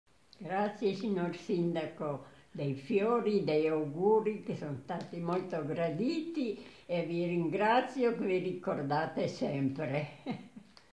PIETRINA LORENZI 102 ANNI RIVA DEL GARDA
Pietrina Lorenzi di Riva del Garda compie 102 anni: ringrazia il sindaco Adalberto Mosaner per gli auguri inviati per il suo compleanno